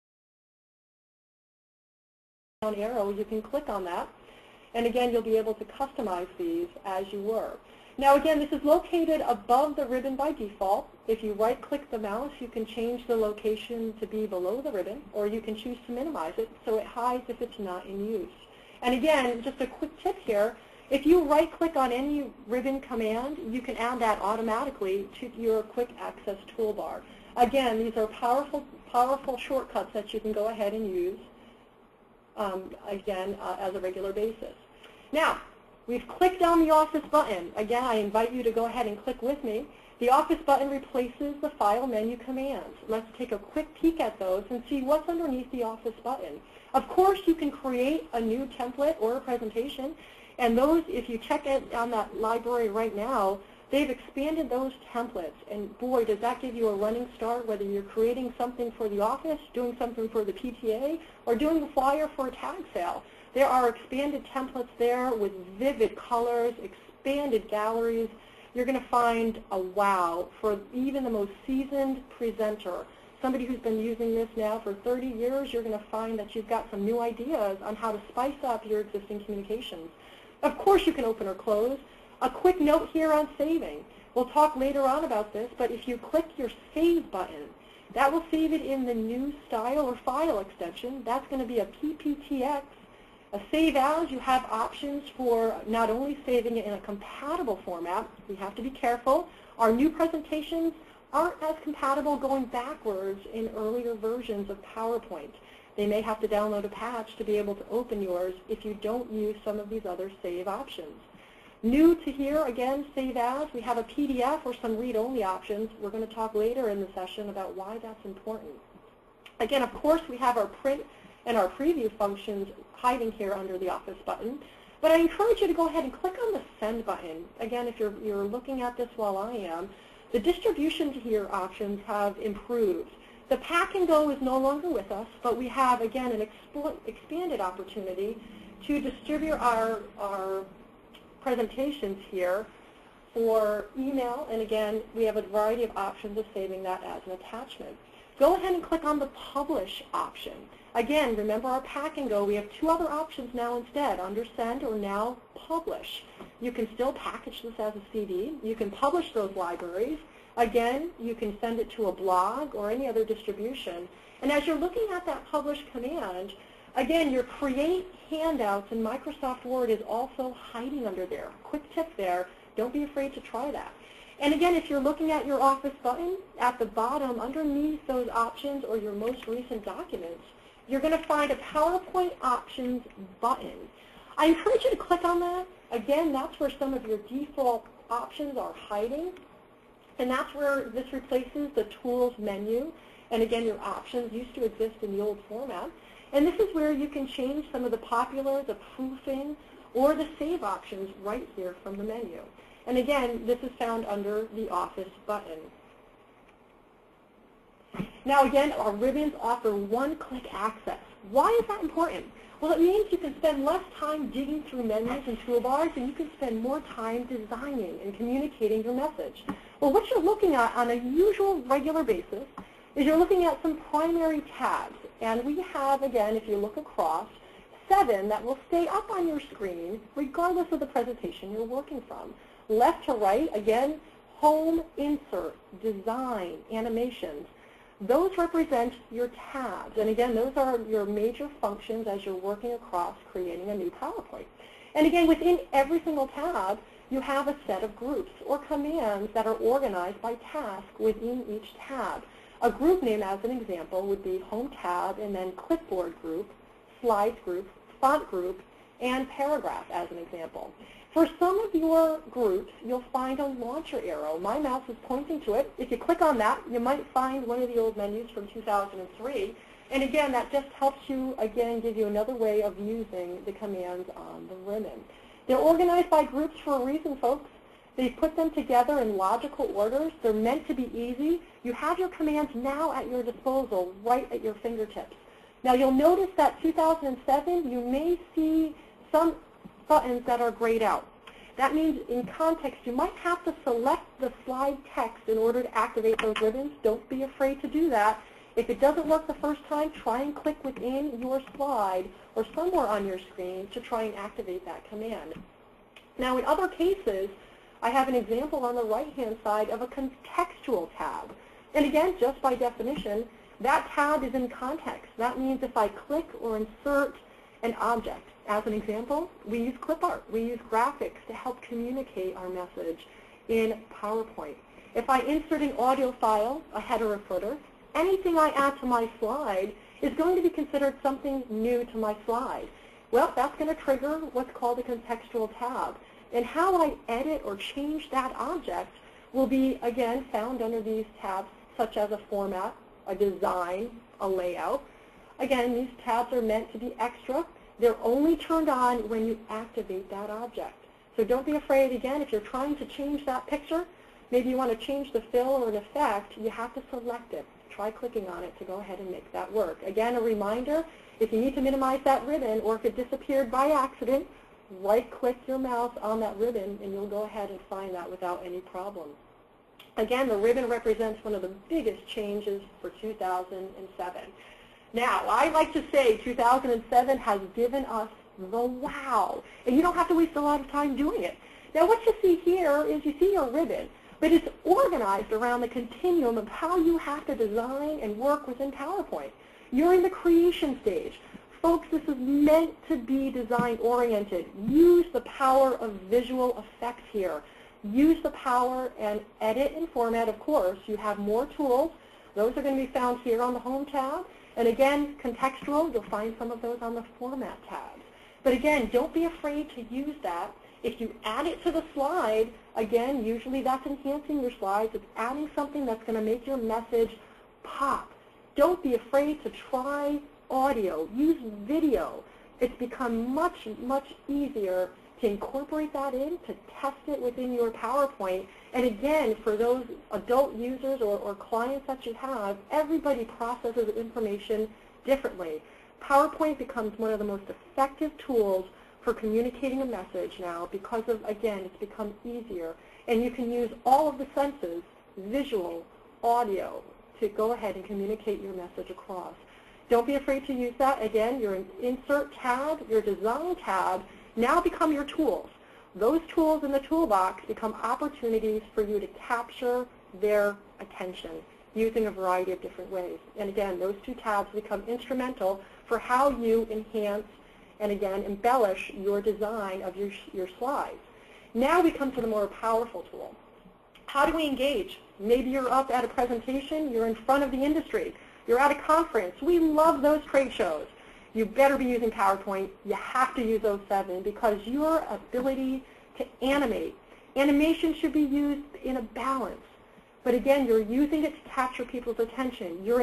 Tips & Tricks PowerPoint 2007 Part 2 of 5
Christine Mansfield of Discovery training shares some of her favorite tips and tricks for PowerPoint 2007.